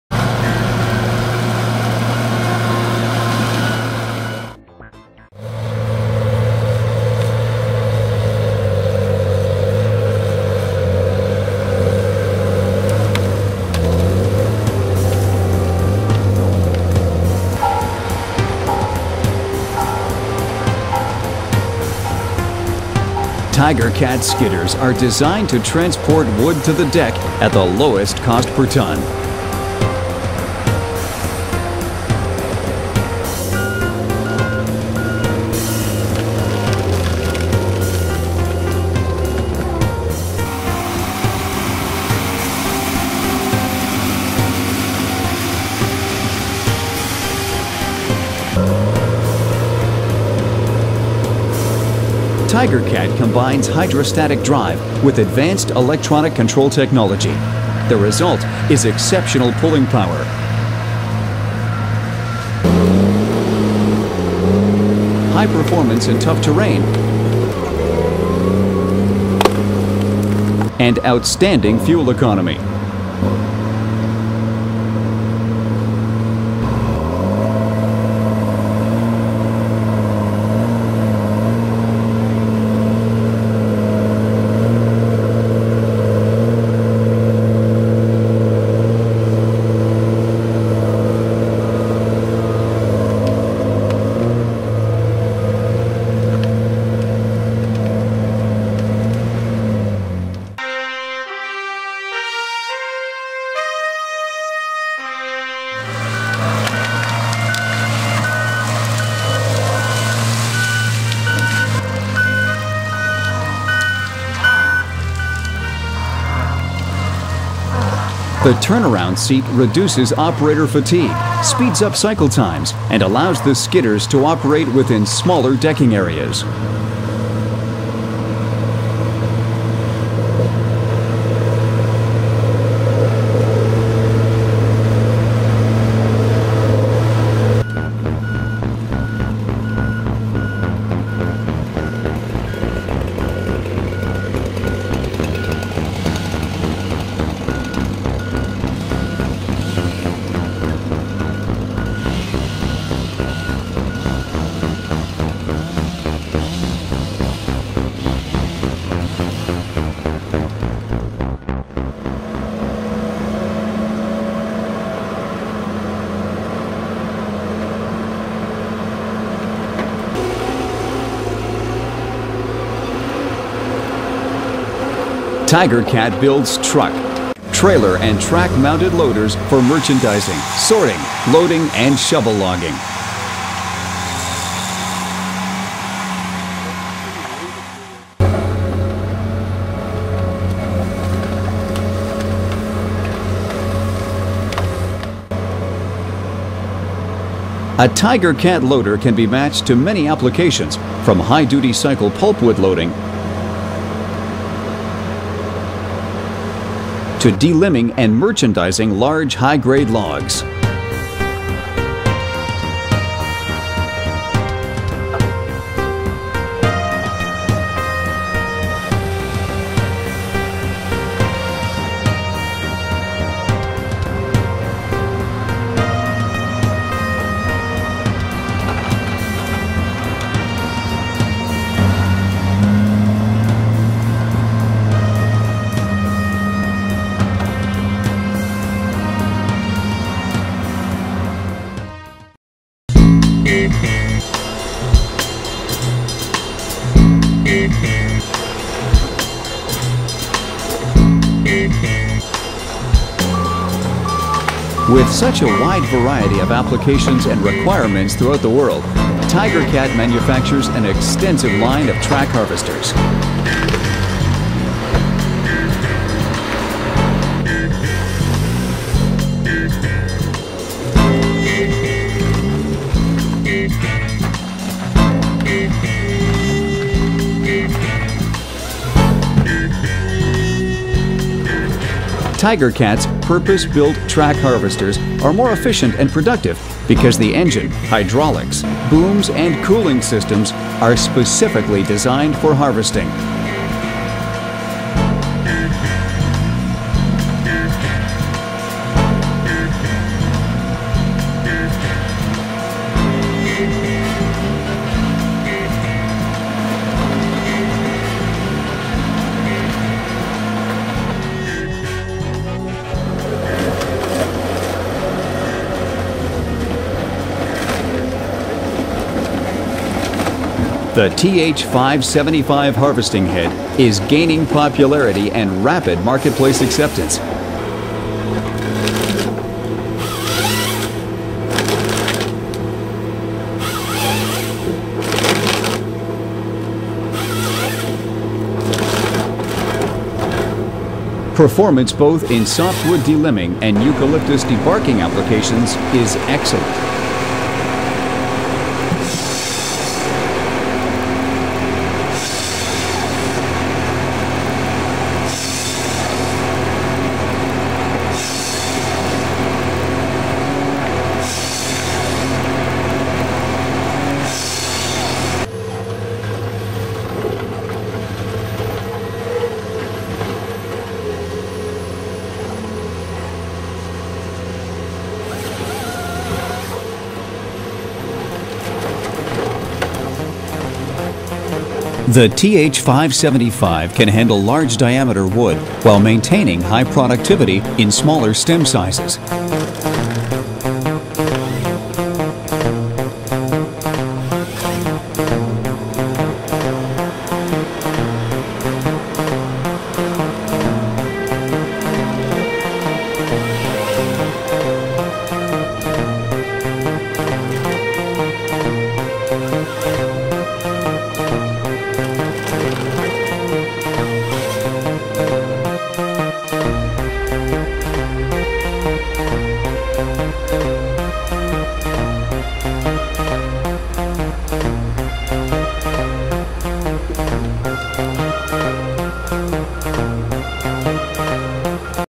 Tigercat 2009 Product video Part2
A brief overview of Tigercat's 2009 product line-up, including tree-length, CTL, mulching and site-prep equipment.